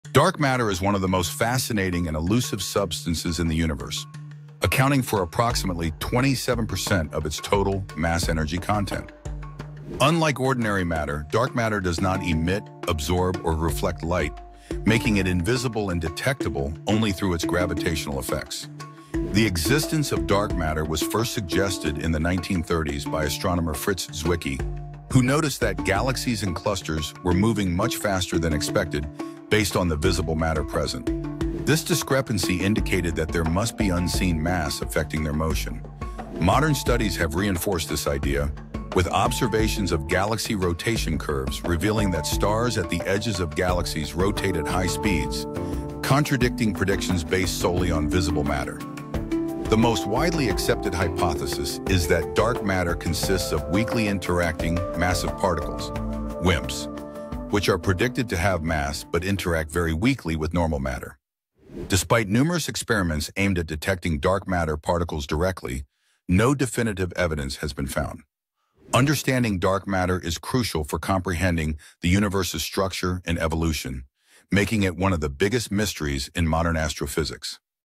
Unveiling Dark Matter's Secrets
What is dark matter? Dive into the mysterious substance that makes up most of the universe's mass. Let's explore its implications in astrophysics!